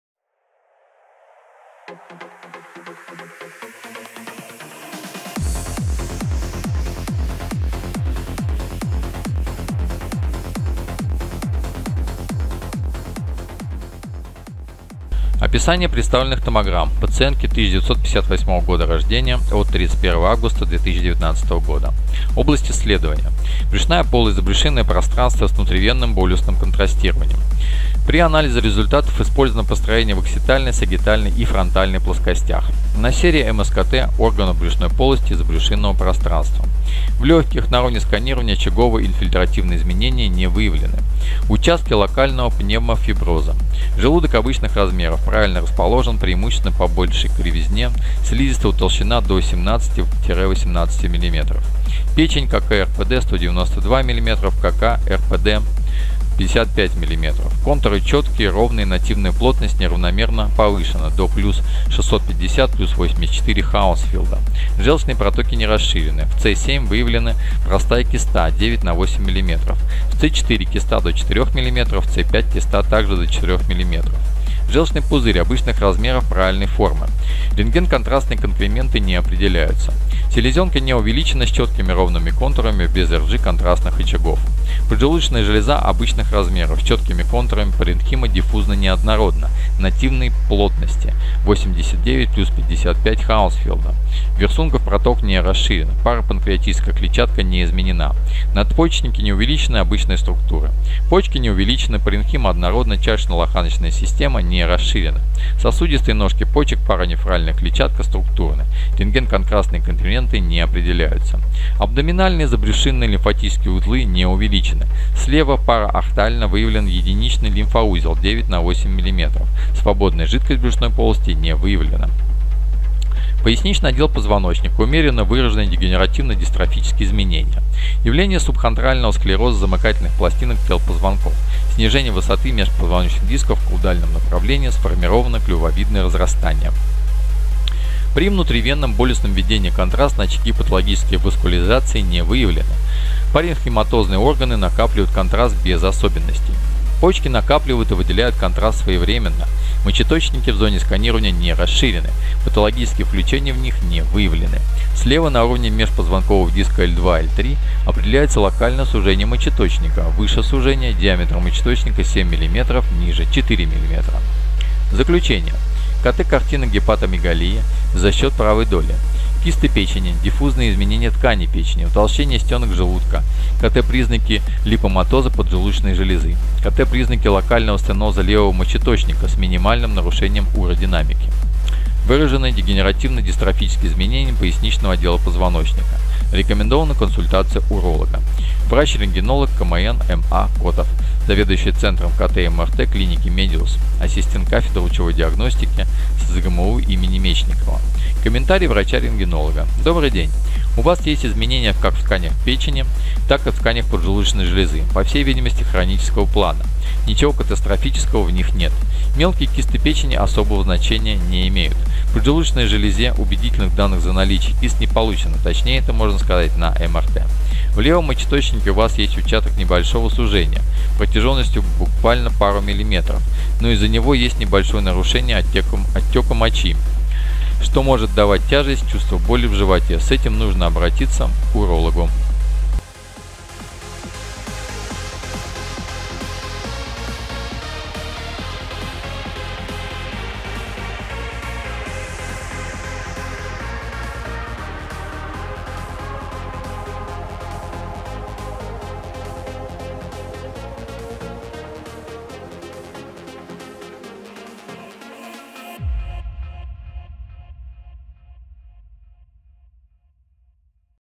Множественные КИСТЫ ПЕЧЕНИ на КТ расшифровке
🔍 Обнаружил множественные КИСТЫ ПЕЧЕНИ на КТ снимках врач-рентгенолог к.м.н. Котов Максим Анатольевич асс. кафедры лучевой диагностики СЗГМУ им. И.И.Мечникова, заведующий Центром КТ и МРТ клиники Медиус.

✔️ Описание представленных томограмм пациентки 1958 г.р. от 31/08/2019.

ОБЛАСТЬ ИССЛЕДОВАНИЯ: Брюшная полость, забрюшинное пространство, с внутривенным болюсным контрастированием

При анализе результатов использованы построения в аксиальной, сагиттальной и фронтальной плоскостях.
На серии МСКТ органов брюшной полости и забрюшинного пространства,

В легких, на уровне сканирования, очаговые и инфильтративные изменения не выявлены. Участки локального пневмофиброза.

Желудок обычных размеров, правильно расположен,  преимущественно по большой кривизне слизистая утолщена до 17-18 мм

Печень  ККРПД - 192 мм, ККРПД - 55 мм.  Контуры четкие, ровные, нативная плотность неравномерно повышена до  +650+84 HU.   Желчные протоки не расширены. В С7 выявлена простая киста 9х8 мм. в С4 киста до 4 мм, в С5 киста так же до 4 мм

Желчный пузырь-  обычных размеров, правильной формы. Рентгенконтрастные конкременты не определяются.
Селезенка не увеличена, с четкими, ровными контурами, без Rg-контрастных очагов.

Поджелудочная железа  обычных размеров, с  четкими контурами, паренхима   диффузно не однородна, нативной плостности - 89+55HU. Вирсунгов проток не расширен. Парапанкреатическая клетчатка  не изменена

Надпочечники не увеличены, обычной структуры.
Почки не увеличены, паренхима однородна, чашечно-лоханочная система не расширена. Сосудистые «ножки» почек, паранефральная клетчатка структурны.   Рентгенконтрастные конкременты не определяются.

Абдоминальные и забрюшинные лимфатические узлы не увеличены.  Слева парааортально выявлен единичный лимфоузел 9х8 мм.
Свободная жидкость  в брюшной полости не выявлена.

Поясничный отдел позвоночника – умеренно выраженные дегенеративно-дистрофические изменения: явления субхондрального склероза замыкательных пластинок тел позвонков, снижение высоты межпозвоночных дисков в каудальном направлении,    сформированы клювовидные разростания.

При внутривенном болюсном введении контраста  - очаги патологической васкуляризации не выявлены. Паринхиматозные органы накапливают контраст без особенностей.

Почки накапливают и выделяют контраст своевременно.   Мочеточники в зоне сканирования не расширены, патологические включения в них не выявлены. Слева на уровне межпозвонкового диска L2-L3 определяется локальное сужение мочеточника - выше сужения - диаметр мочеточника 7 мм, ниже 4 мм.

✔️ЗАКЛЮЧЕНИЕ: КТ - картина гепатомегалии, за счет правой доли. Кисты печени. Диффузные изменения ткани печени. Утолщение стенок желудка. КТ - признаки липоматоза поджелудочной железы. КТ - признаки локального стеноза левого мочеточника, с минимальным нарушением уродинамики.
Выраженные дегенеративно-дистрофические изменения поясничного отдела позвоночника. Рекомендована консультация уролога.

Врач-рентгенолог, к.м.н. М.А. Котов
Заведующий Центром КТ и МРТ клиники Медиус.Ассистент кафедры лучевой диагностики СЗГМУ им. И.И. Мечникова.

✅ Комментарий врача-рентгенолога:
"Добрый день.
У Вас есть изменения как в тканях печени, так и в тканях поджелудочной железы - по всей видимости хронического плана, ничего катастрофического в них нет.

Мелкие кисты печени особого значения нет.
В поджелудочной железе убедительных данных за наличие кист не получено, точнее это можно сказать на МРТ.

В левом мочеточнике У Вас есть участок небольшого сужения, протяженностью буквально пару миллиметров, но из-за него есть небольшое нарушение оттока мочи, что может давать тяжесть, чувство боли в животе.
С этим нужно обратиться к урологу."

ГЕПАТОМЕГАЛИЯ – синдром, характеризующийся увеличением размеров печени.

☑️ ПРИЧИНЫ гепатомегалии:
➖ сердечно-сосудистые заболевания.
➖ вирусные гепатиты;
➖ жировая дистрофия;
➖ инфекционный мононуклеоз;
➖ алкоголизм.

👨‍👩‍👧‍👧 Тщательно изучите ОТЗЫВЫ ПАЦИЕНТОВ, получивших расшифровку КТ брюшной полости за 1500 руб. в течение 4-х ч.